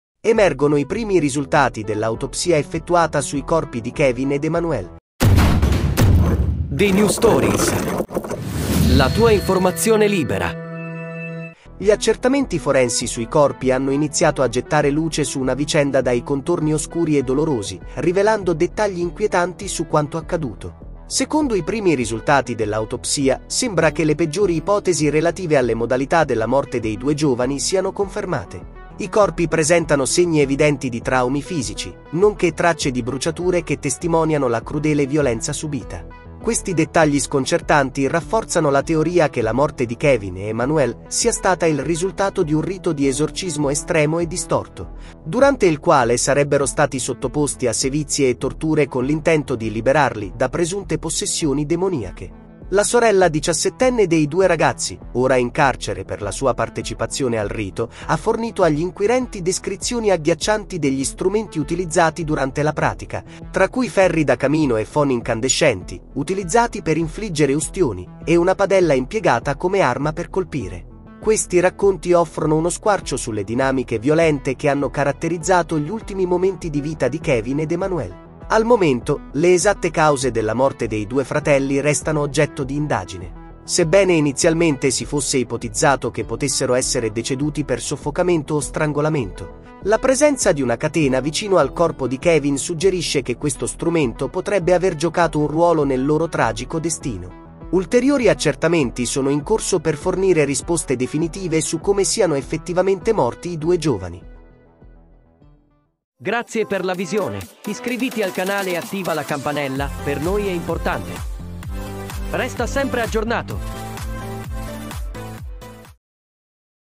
STRAGE ALTAVILLA MILICIA: RISULTATI AGGHIACCIANTI DALL'AUTOPSIA SUL CORPO DI KEVIN ED EMMANUEL
Strage di Altavilla, risultati agghiaccianti dell’autopsia sui corpi di Kevin ed Emanuel

❗IMPORTANTE❗
PER RICHIESTE DI COLLABORAZIONI PUBBLICITARIE:
invia una mail  e verrai contattato il prima possibile: